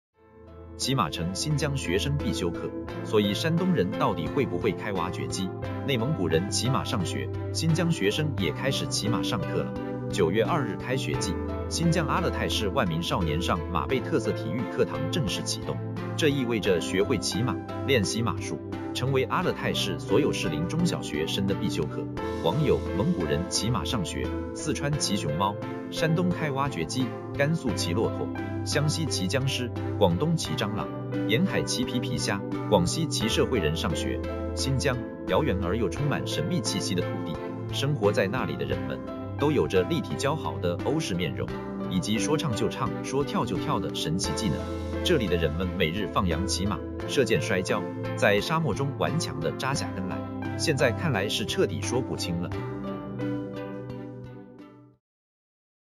骑马成新疆学生必修课 所以山东人到底会不会开挖掘机
➤Thanks收看！